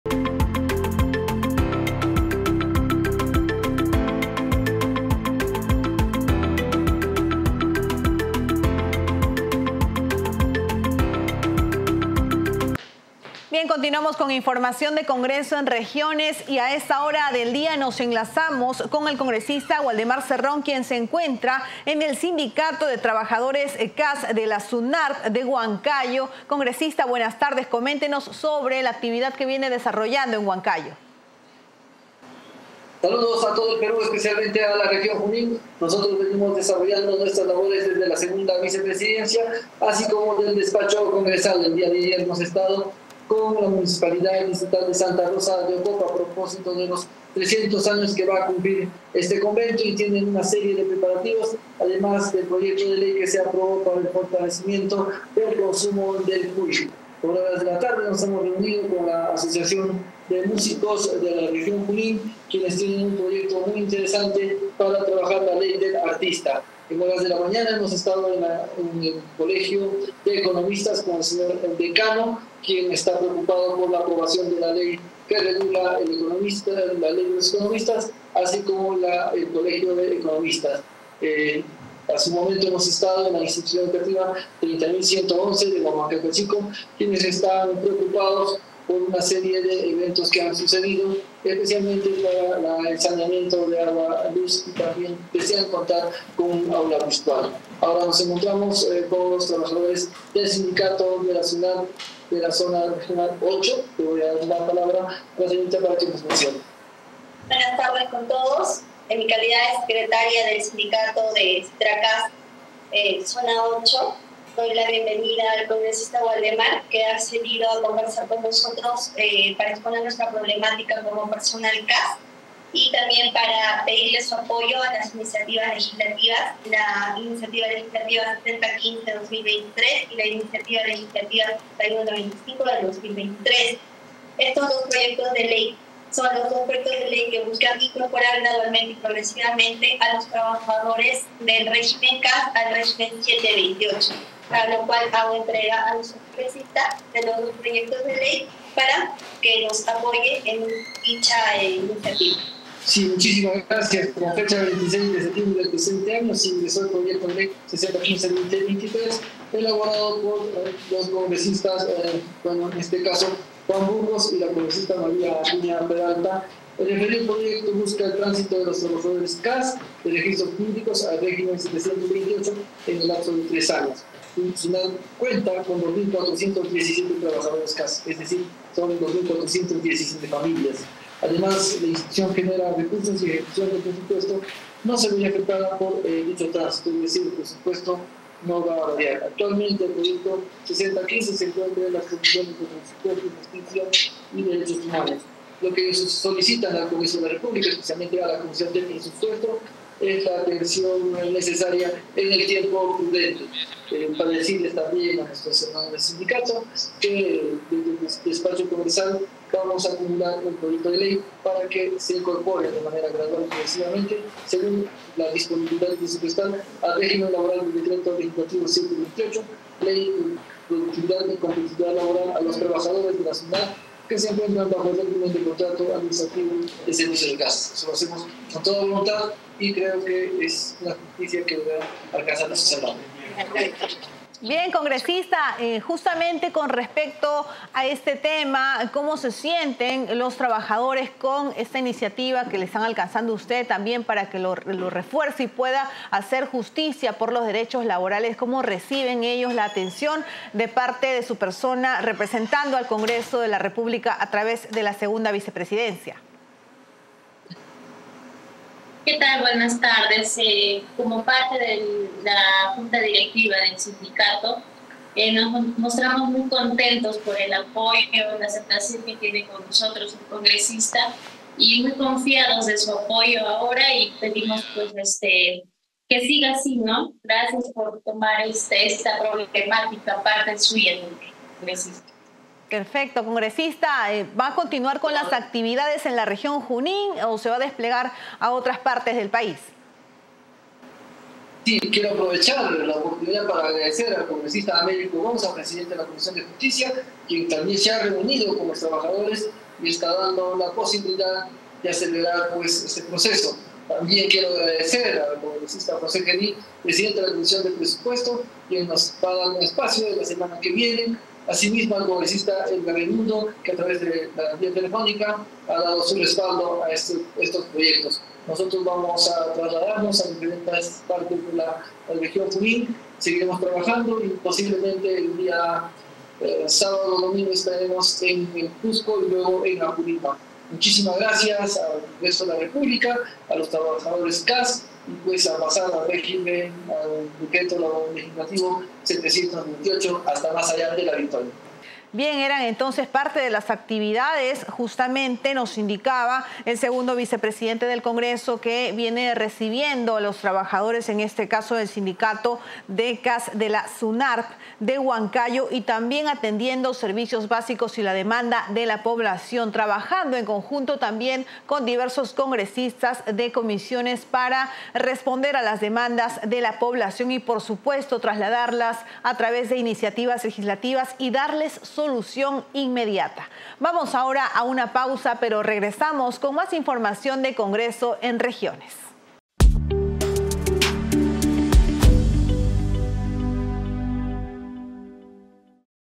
Comprometidos con proyectos de ley para beneficiar a trabajadores CAS
CongresoEnRegiones | Como parte de la semana de representación, el segundo vicepresidente del Congreso, Waldemar Cerrón, se reunió con representantes en el Sindicato de Trabajadores Cas de la Sunarp en Huancayo, región Junín.

Canal oficial del Congreso de la República del Perú.